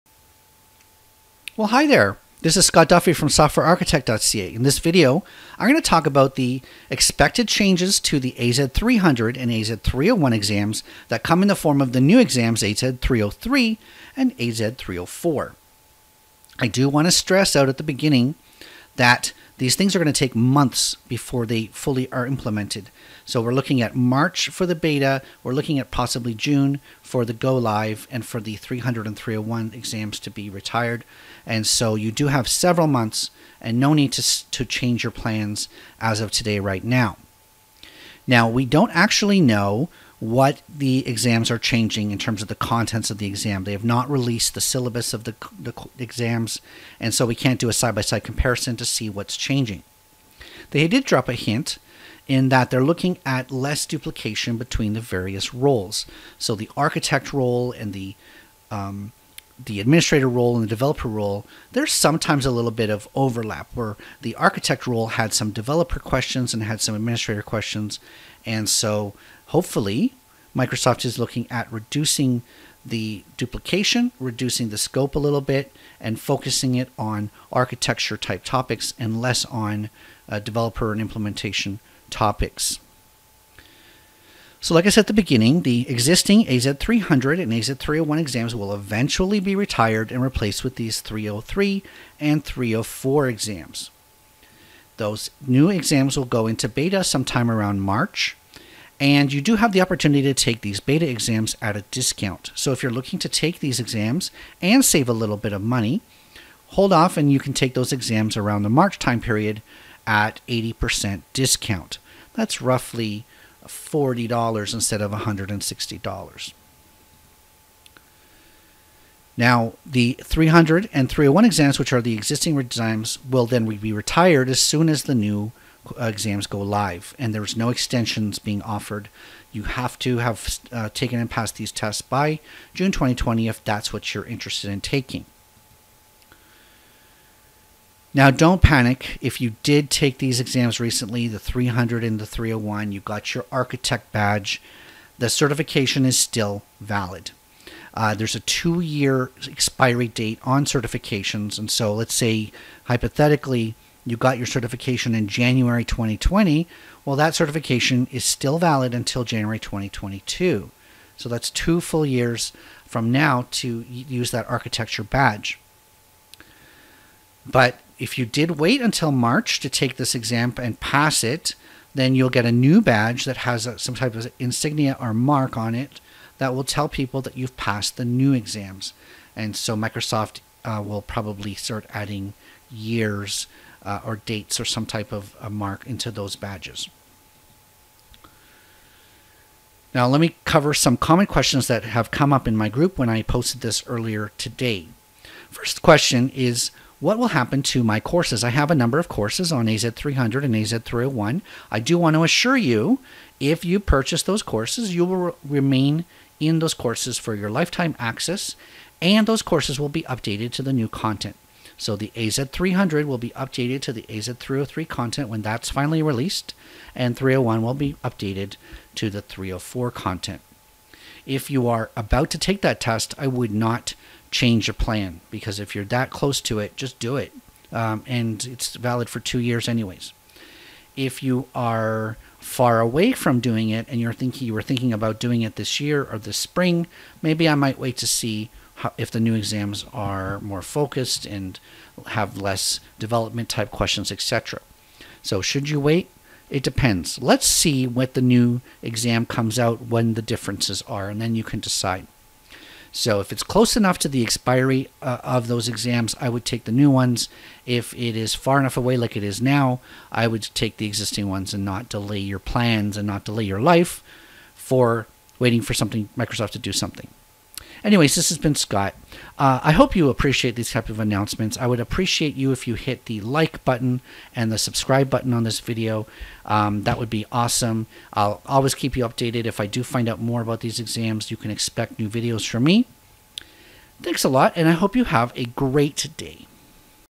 New AZ-303 and AZ-304 Exams Replace AZ-300 and AZ-301 - What We Know!
Today's announcement of the new exam codes coming in March 2020 has some interesting clues.

In this video, I go over what we know for AZ-303 and AZ-304.